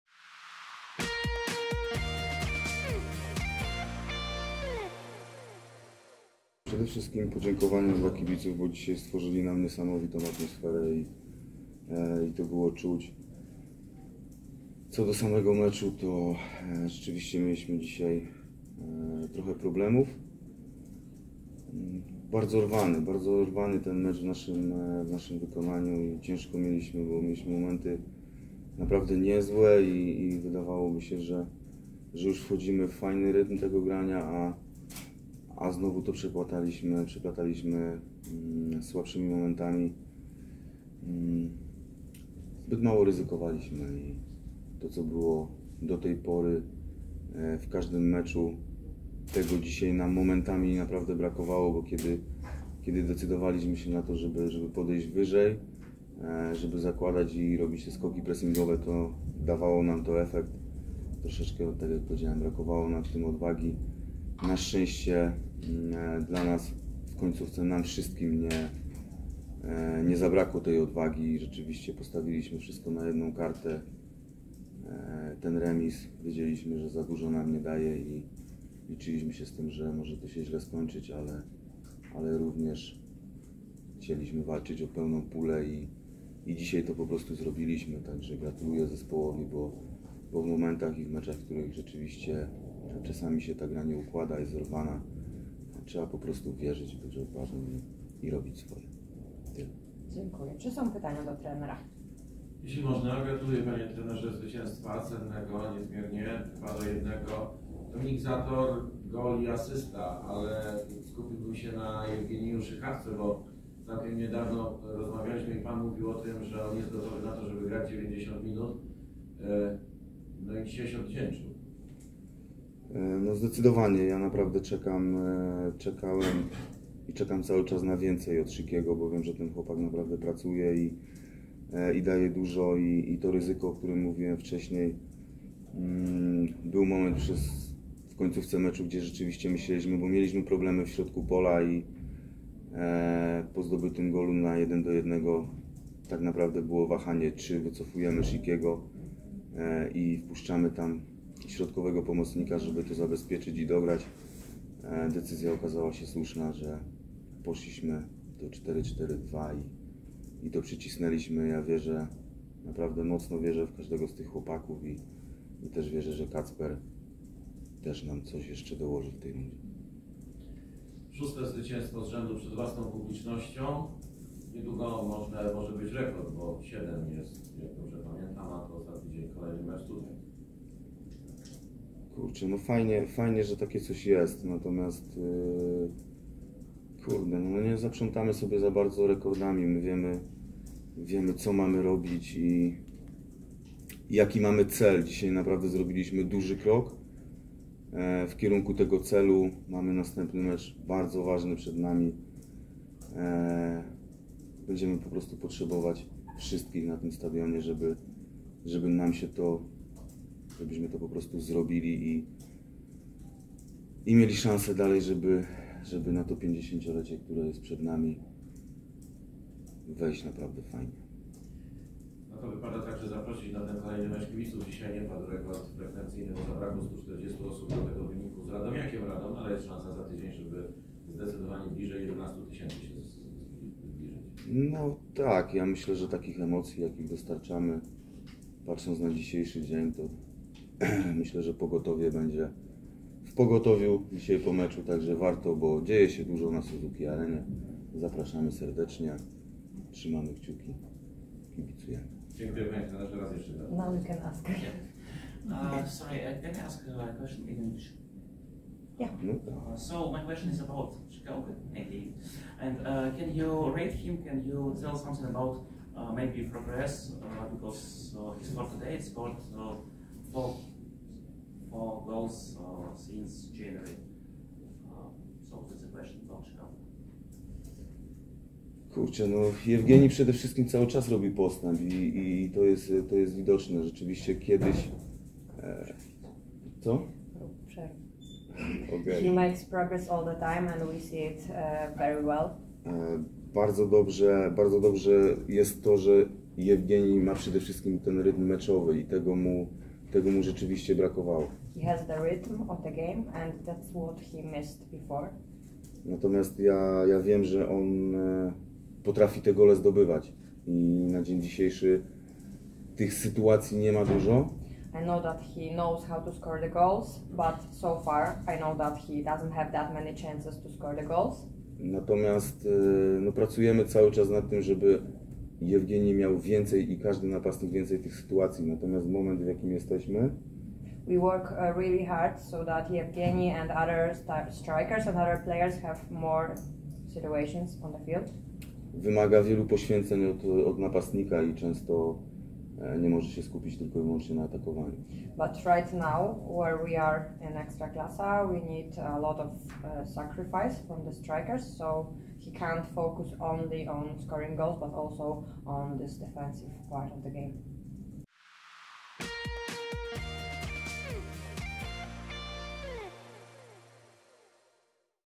Kamil Kuzera po meczu Korona Kielce - Jagiellonia Białystok (2:1), 15.04.2023
Piłkarze Jagiellonii Białystok przegrali wyjazdowe spotkanie z Koroną Kielce 1:2, mimo, że przez większą część meczu prowadzili 1:0. Białostoczanie stracili decydującego gola w 95. minucie meczu.

Znajdź i słuchaj naszych podcastów na Spotify i Google Podcasts.